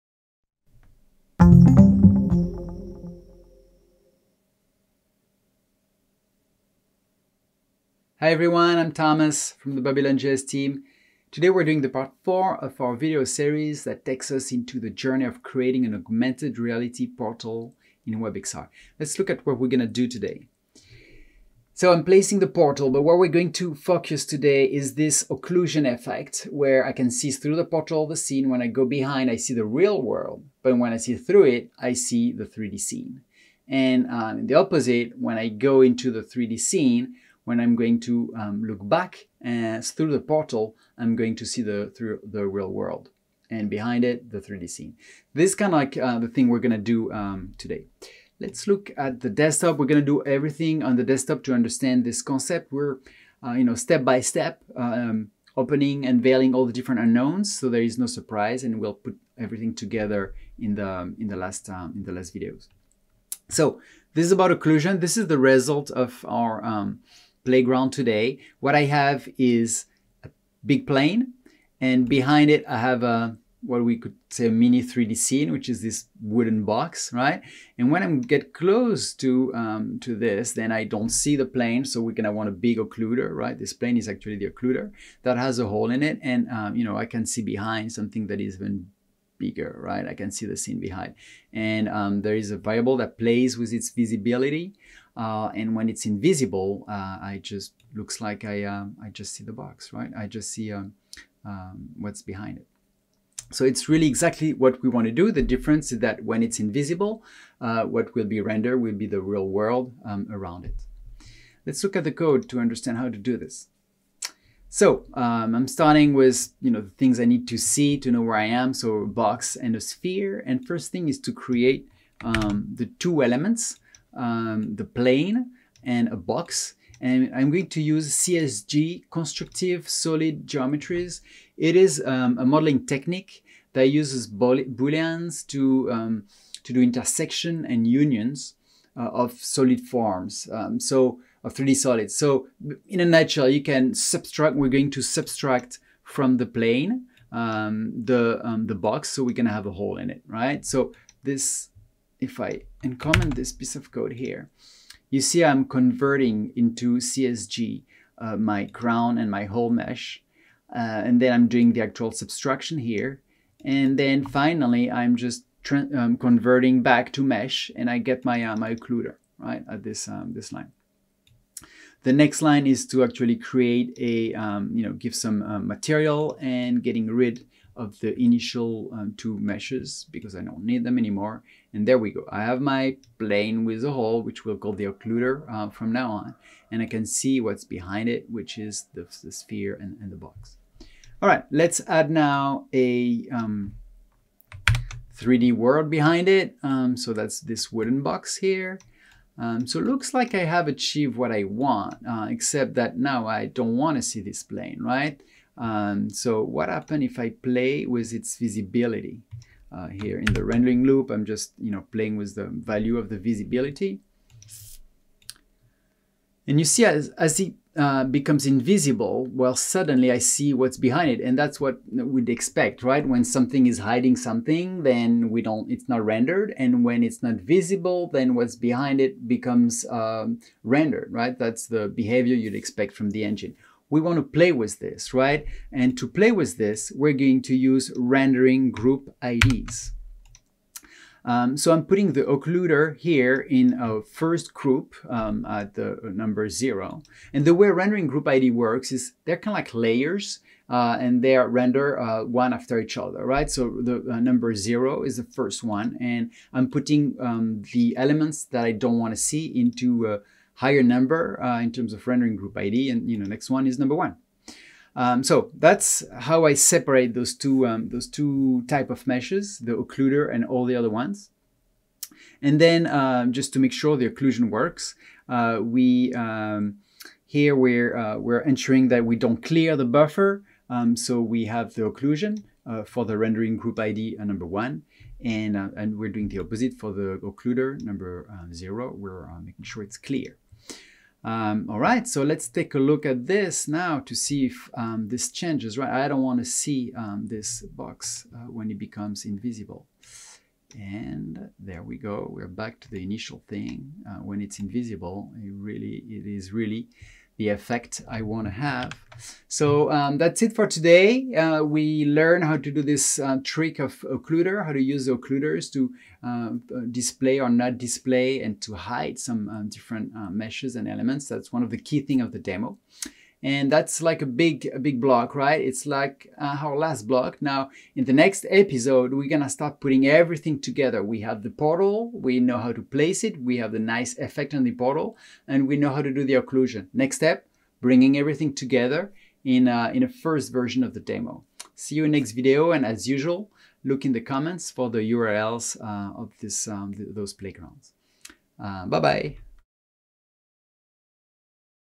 Beginner Series - AR Portal Pt4: Occlusion Rendering
In this series, which is for beginners, Thomas takes us step by step through the journey of creating an Augmented Reality Portal with WebXR and Babylon.js. You'll explore all the building blocks of this demo (object placement in WebXR, visual effects, occlusion rendering) before bringing them all together into the final demo.

In Part 4, you’ll learn how to create the portal and add some cool visual effects using particle systems and glowing effect

Links used in this Part 4:

– Code commented during the video: occlusion rendering.